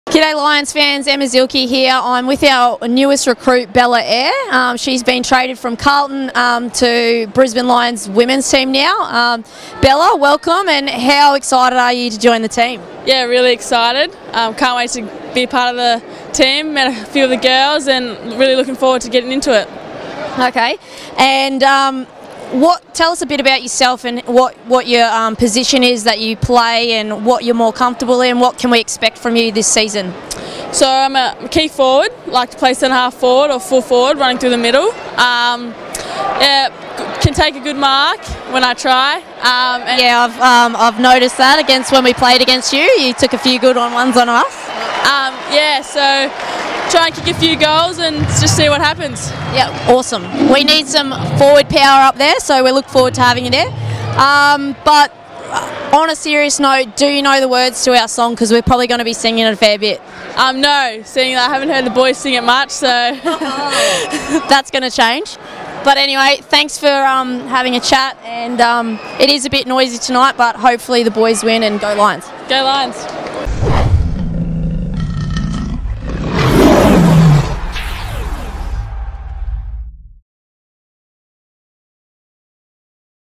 AFLW new recruit: Isabella Ayre
Captain Emma Zielke catches up with new recruit Isabella Ayre at the Gabba on Saturday night.